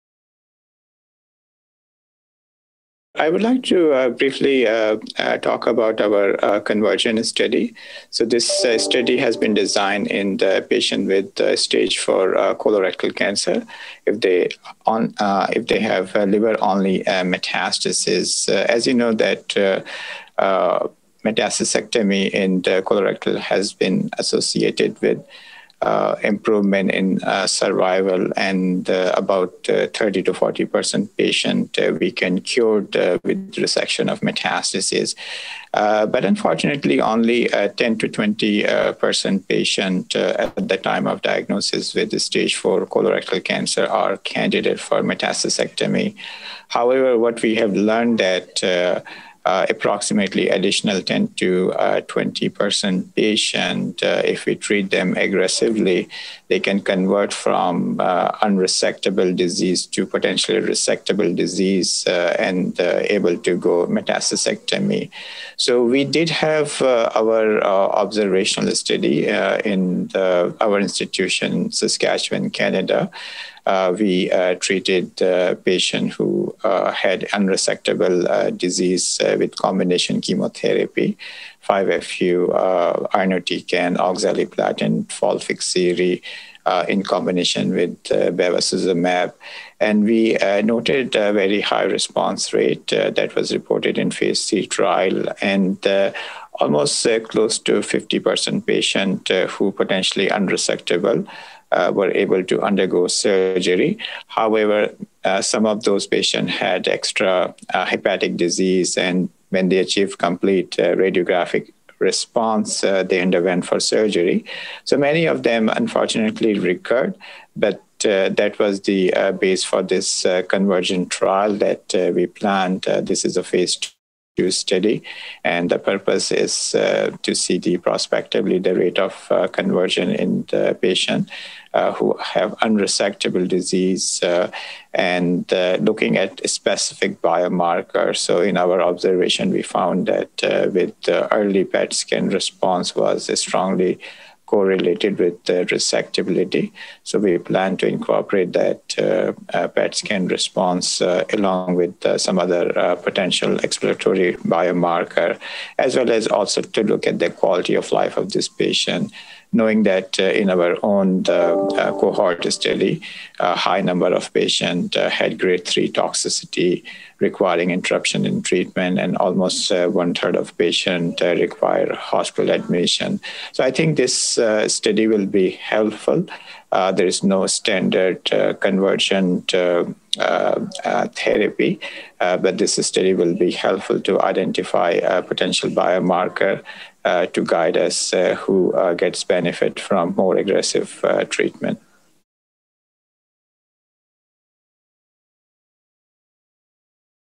CONVERSION from unresectable to resectable mCRC in real-world patients
Shahid Ahmed, PhD, MD, FRCPC, FACP, Saskatoon Cancer Center, University of Saskatchewan, Saskatoon, Canada, discussing the ongoing CONVERSION trial (NCT03401294), a pragmatic Phase II study aiming to assess the rate of conversion therapy in real-world patients with unresectable, liver-only metastatic colorectal cancer (CRC) following the combination of FOLFOXIRI and bevacizumab. The trial in progress, presented at the American Society of Clinical Oncology (ASCO) Gastrointestinal Cancers Symposium 2021, also aims to determine the role of various biomarkers, including an early FDG-PT/CT response, in predicting curative hepatic metastasectomy. This interview was recorded via an online conference call with The Video Journal of Oncology (VJOncology).